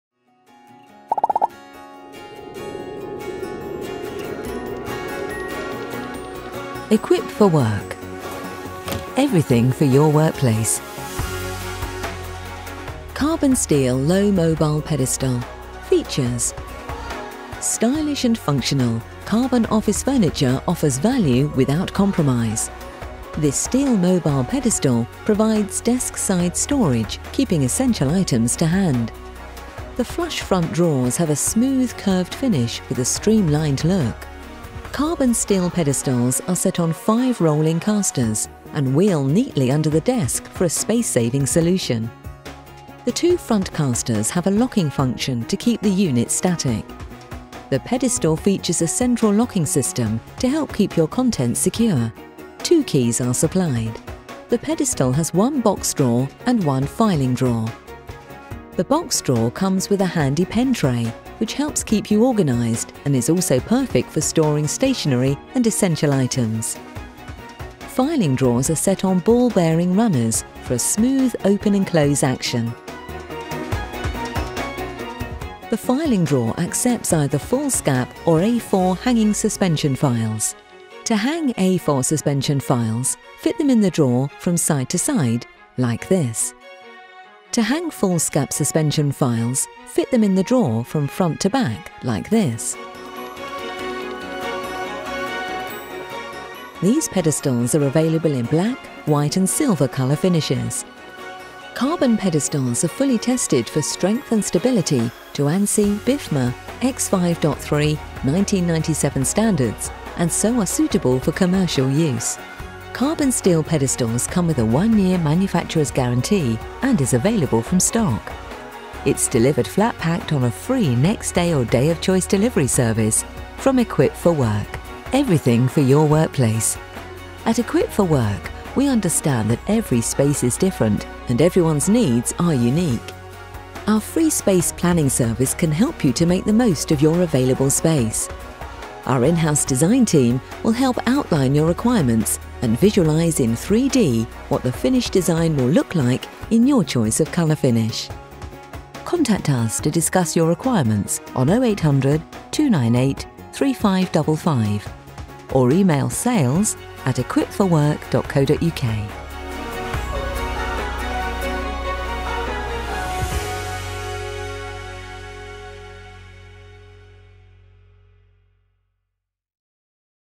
Karbon Steel Mobile Low Mobile Pedestal - Features
Familiarise yourself with the features and functions of the Karbon Steel Mobile Low Mobile Pedestal from Equip4Work with this video.

These low mobile steel pedestals are quality guaranteed, and feature 1 stationery drawers and 1 filing drawer. Available in black, white or silver.

FREE NEXT DAY UK Mainland delivery direct from our UK based warehouse.